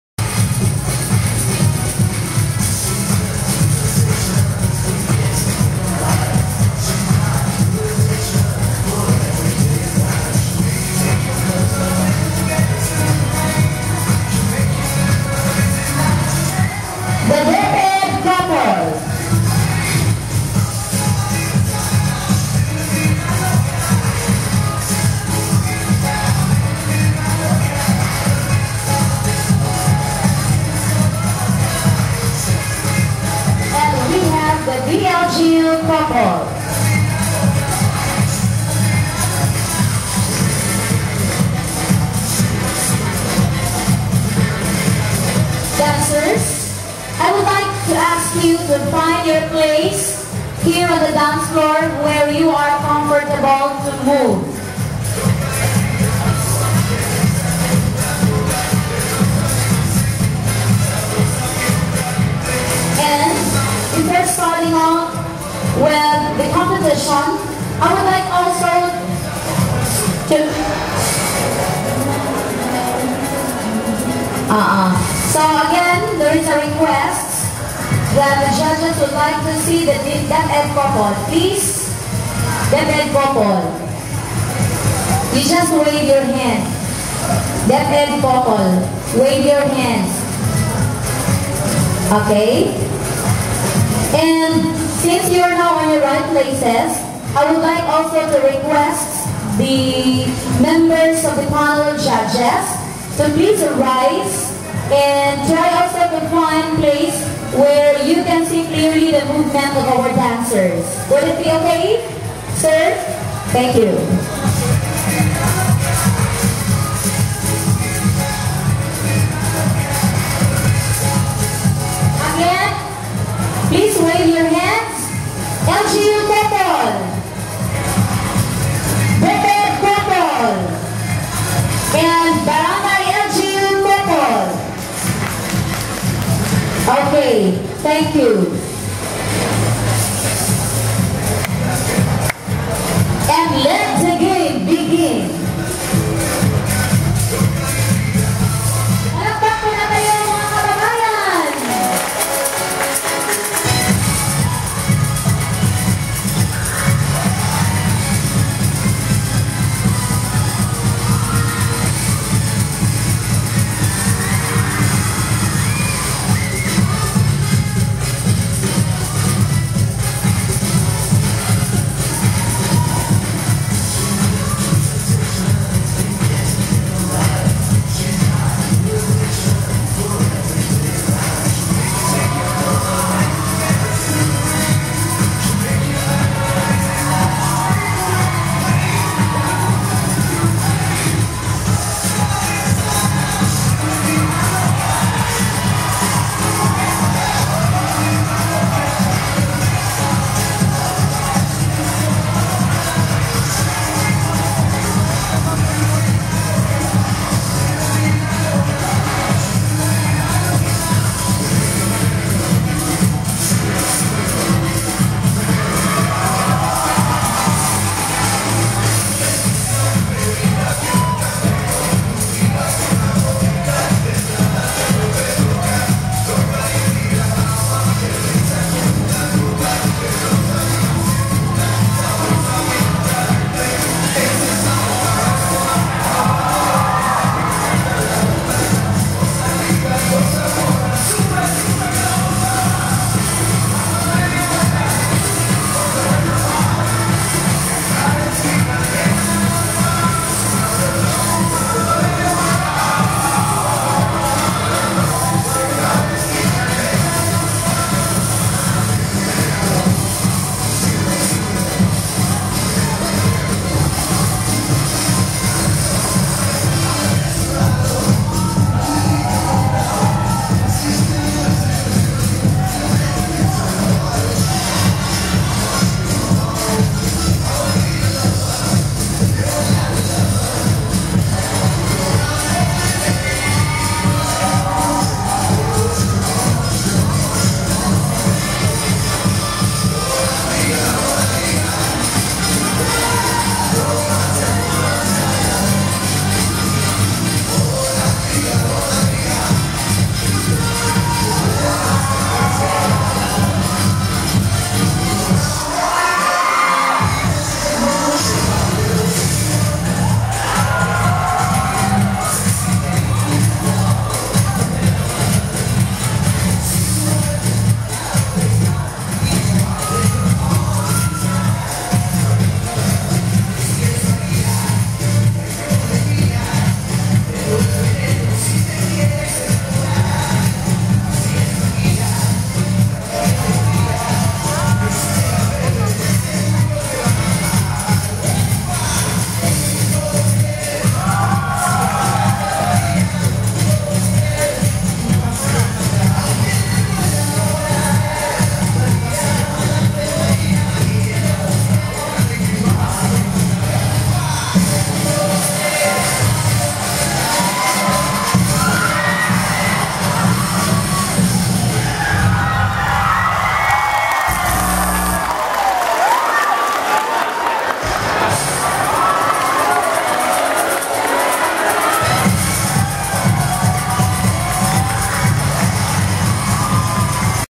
Tabina@51_Unitynight_ep7
A ballroom showdown between the DepEd Couple, LGU Couple, & BLGU Couple during the 51st Araw Ng Tabina Celebration last August 16, 2012.